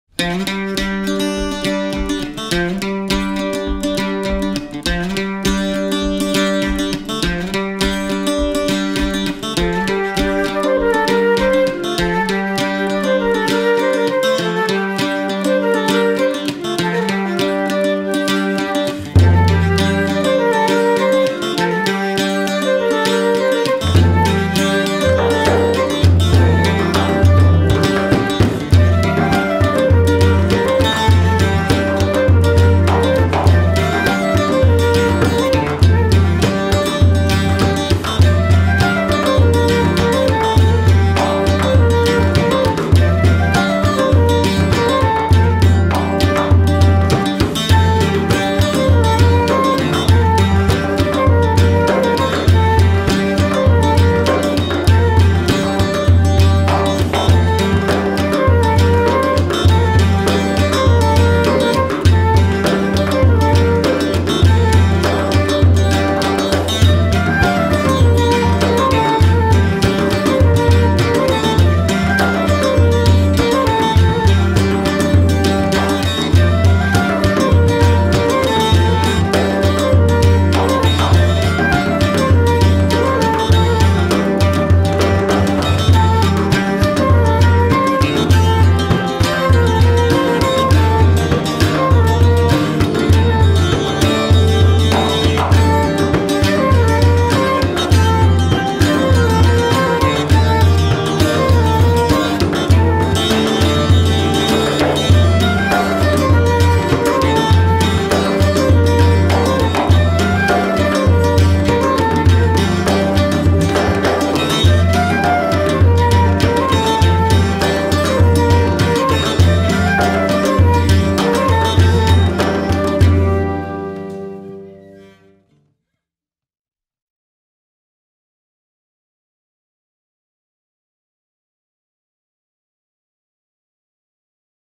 Déambulation et parades "Le peuple de Moriquendi"
La Compagnie Moriquendi vous présente sa déambulation féerique "Le peuple de Moriquendi".
Parades d'échassiers, échassières, jongleurs et final en option avec  le tissu aérien !